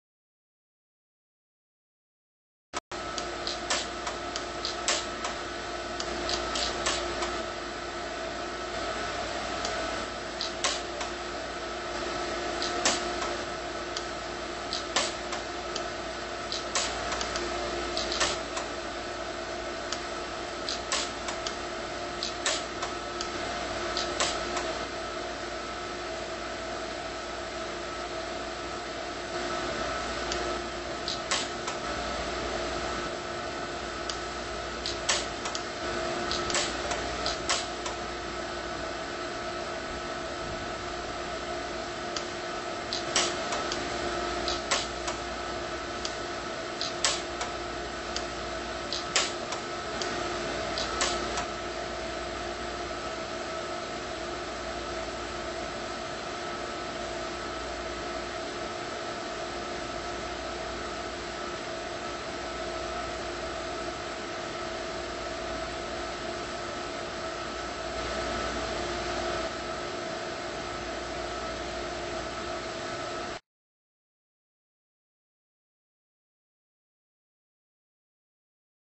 Pulse Arc Weld 28mil Silver Jump Ring to 28mil Silver Plate with Orion Welder
Welder:
Orion Pulse 150i - Pulse Arc and Resistance Welder

The Orion Pulse 150i is the best choice for welding silver of any size.